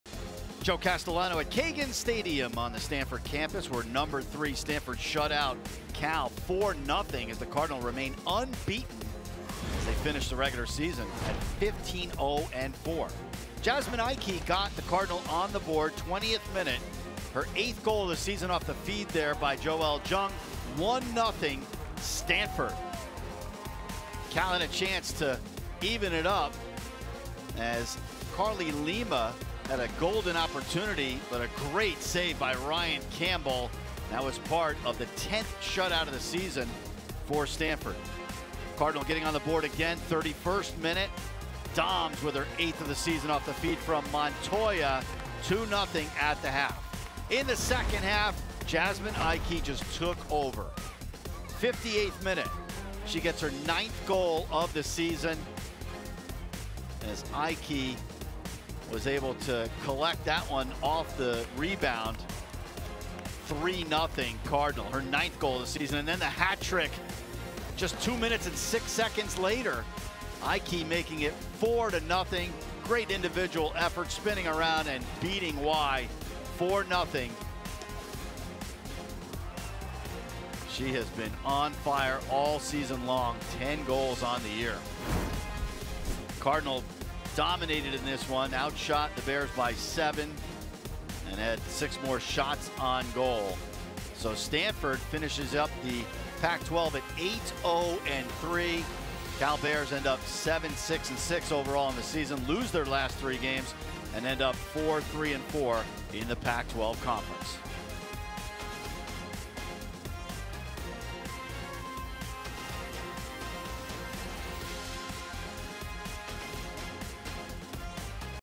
No. 3 Stanford vs. California Women's Soccer Highlights | 2023 Season
No. 3 Stanford women's soccer defeated California by a final score of 4-0 on Friday, Nov. 3 at Cagan Stadium. The Cardinal closed regular season play at 15-0-4 overall and 8-0-3 in Pac-12 play behind a hat-trick from Jasmine Aikey and Maya Doms' 8th goal of the season.